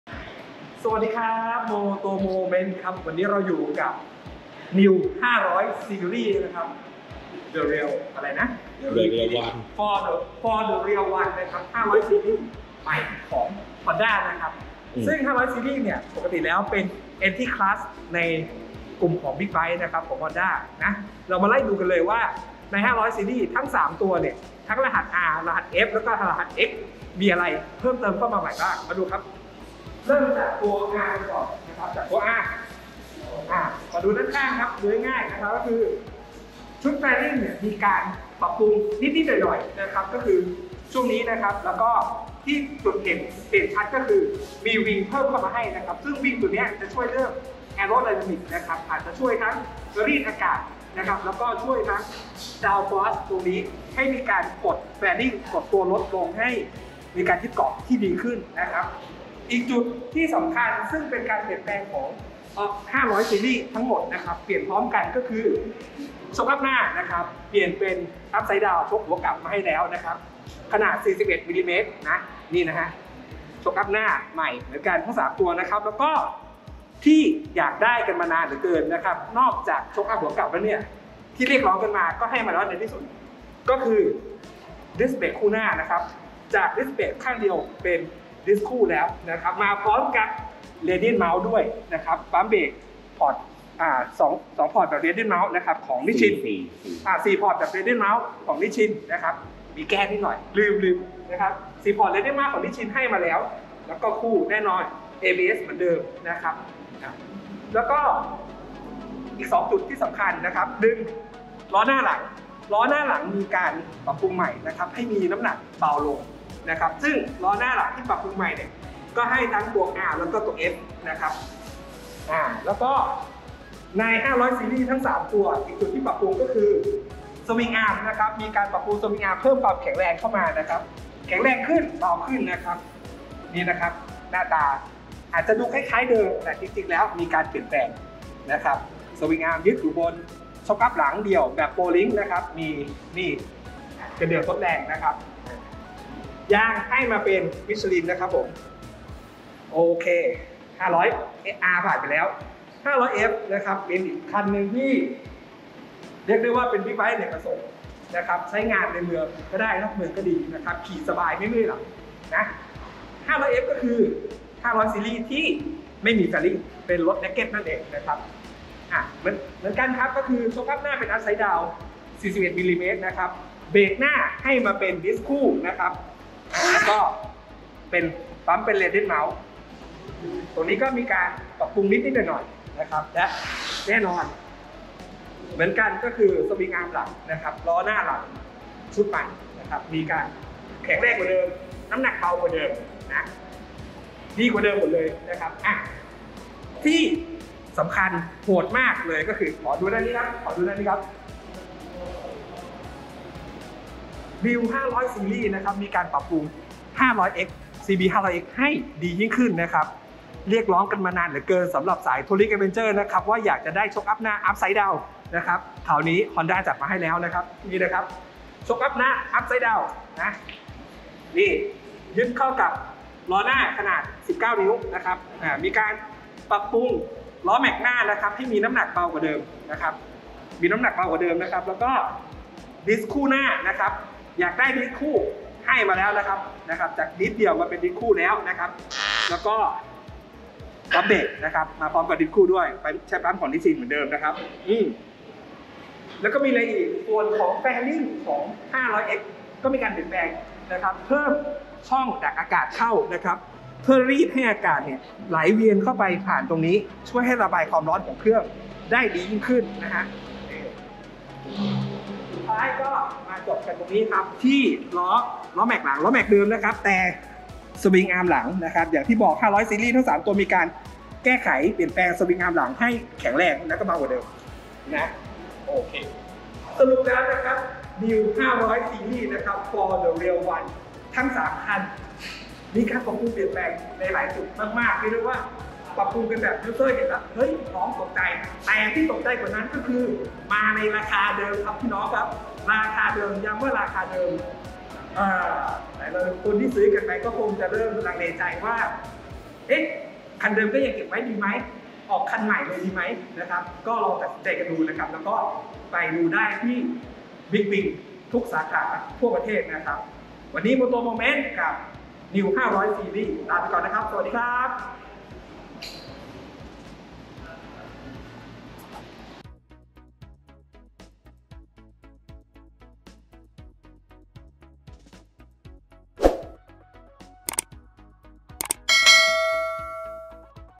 PREVIEW NEW 500 SERIES 2022 CBR500R / CB500F / CB500X พรีวิวฮอนด้า500ใหม่ล่าสุดทั้งสามรุ่น
PREVIEW NEW 500 SERIES CBR500R / CB500F / CB500X พรีวิวฮอนด้า500ใหม่ล่าสุดทั้งสามรุ่นทั้งรหัส R/F/X มีอะไรใหม่ตรงไหน ดีมั้ยที่เพิ่มมาให้กับราคาที่ไม่เพิ่มจากเดิม